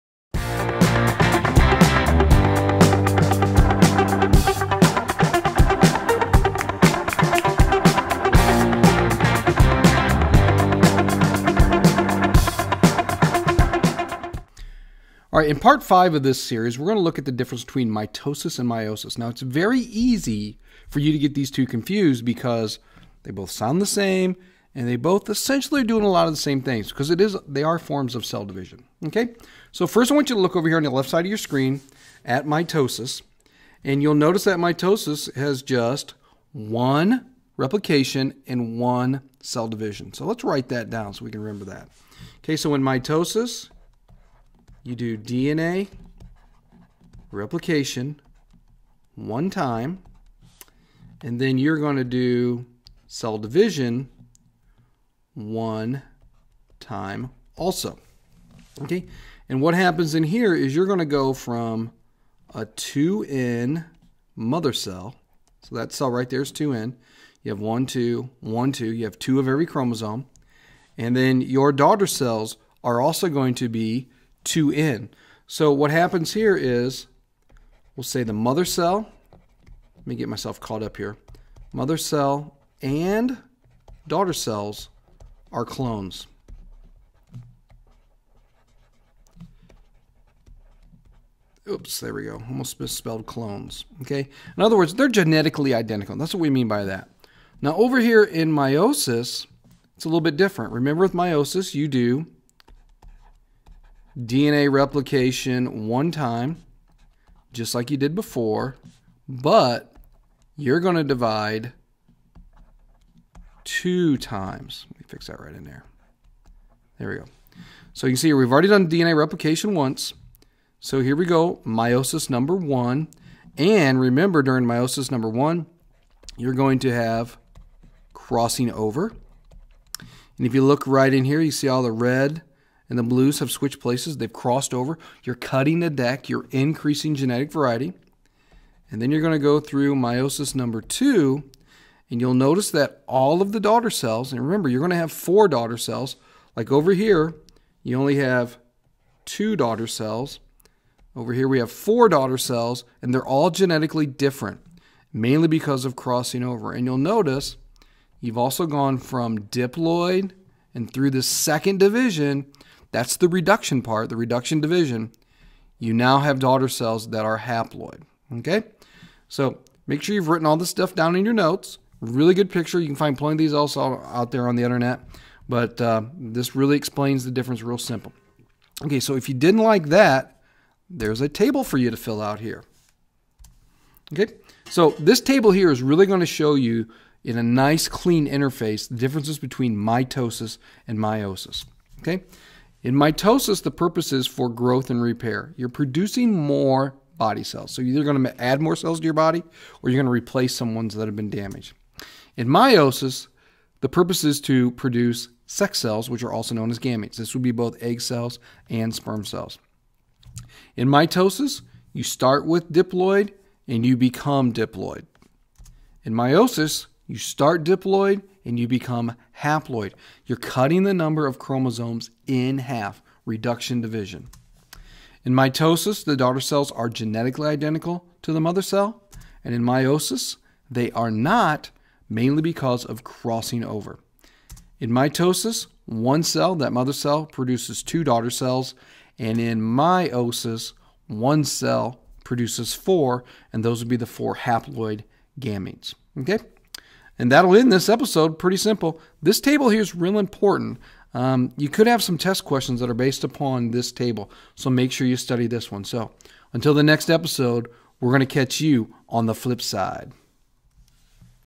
Chapter 11 Part 5 - Mitosis vs. Meiosis
This episode compares the similarities and differences between mitosis and meiosis.